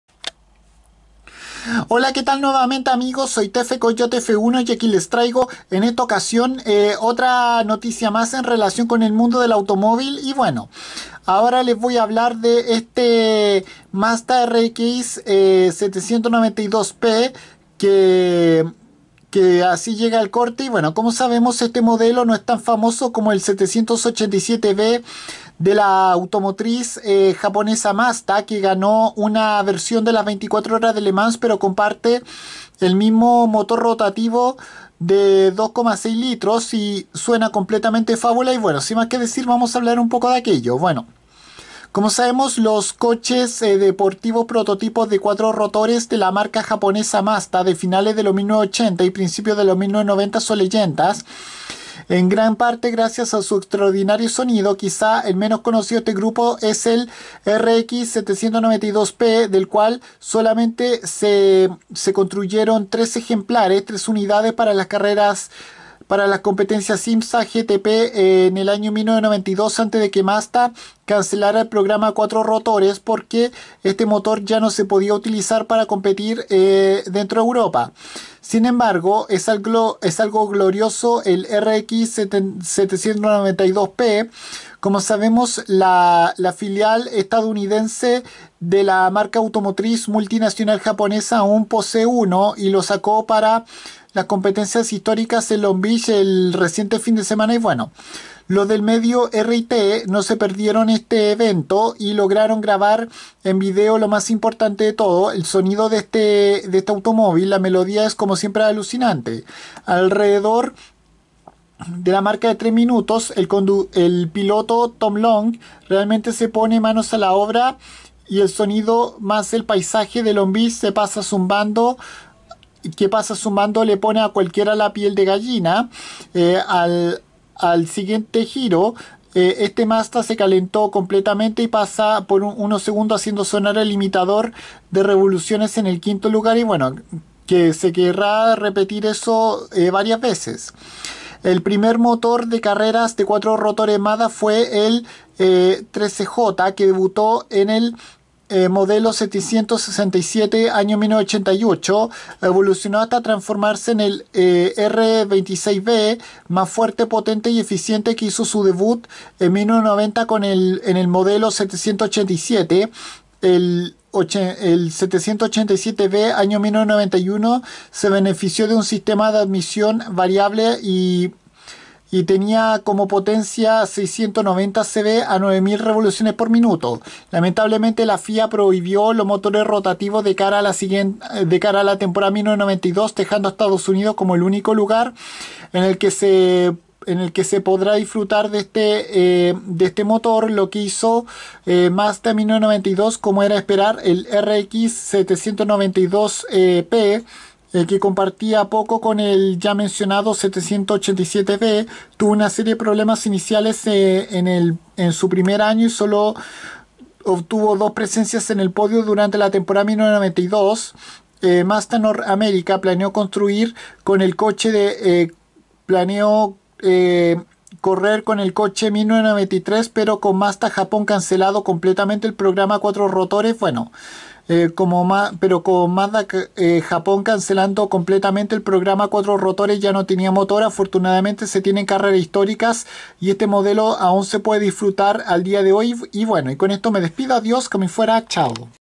Así llega a corte este Mazda RX792P
Este coche prototipo de carreras japonés no es bastante conocido como el ganador de las 24 Horas de Le Mans 787B pero posee el mismo motor rotativo de 2.6 litros de cilindrada